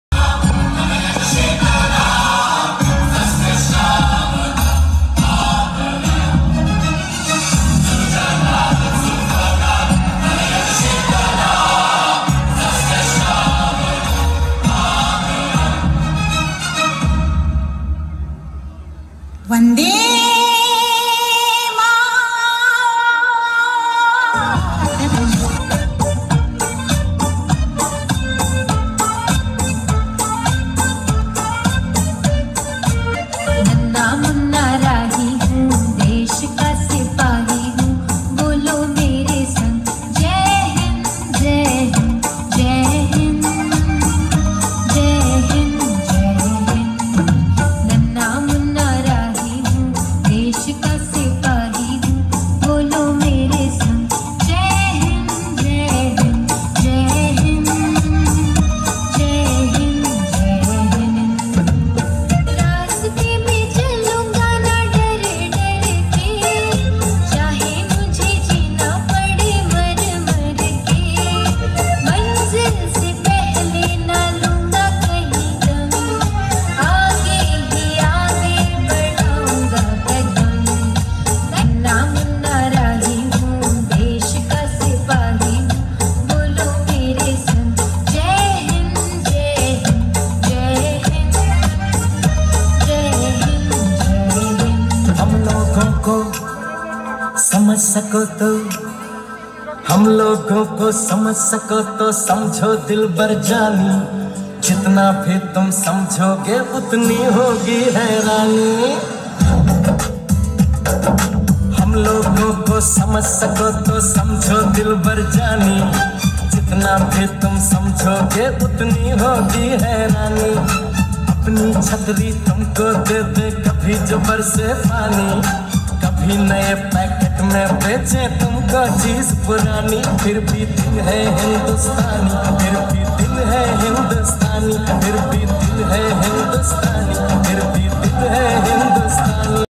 Kids dancing on Indian Patriotic medlies at the India Day Festival 2018 on Aug 19th
Kids dancing on Indian Patriotic medlies at the India Day Festival 2018 on Aug 19th. The event was organized by ICO, India Canada Organization.